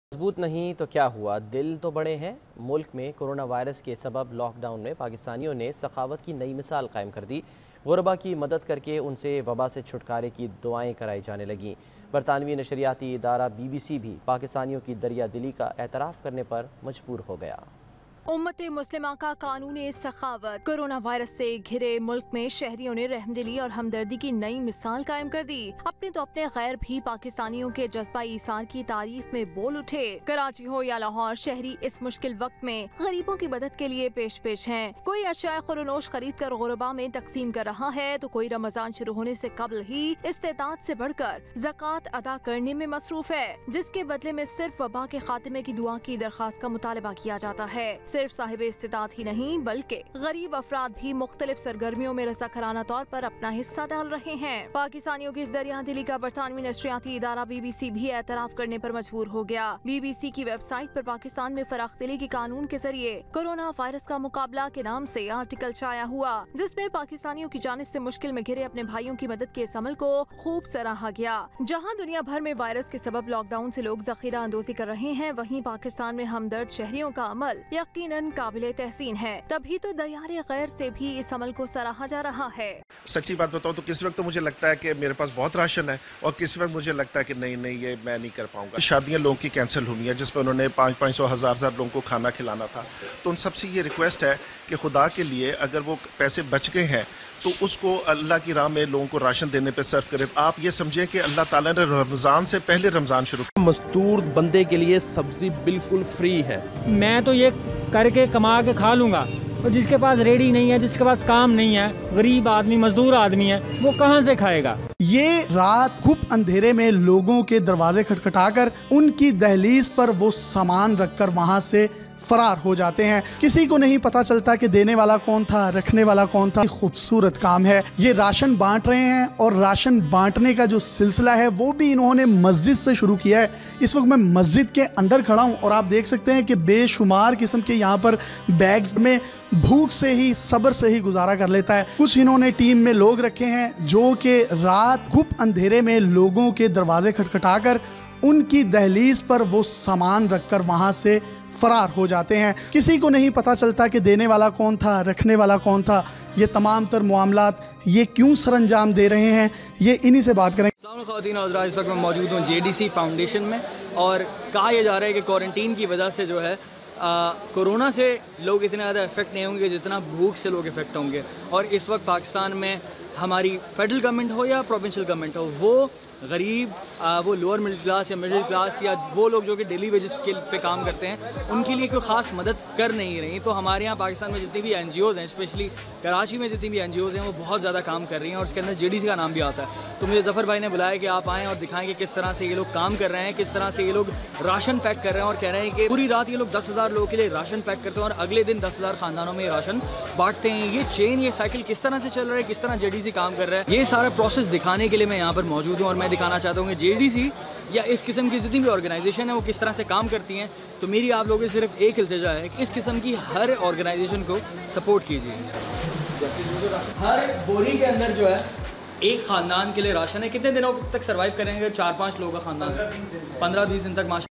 The law of generosity combatting coronavirus in Pakistan, Covid-19 pandemic, BBC
Have a complete look at the video to find out more about the donations given by the needy persons themselves.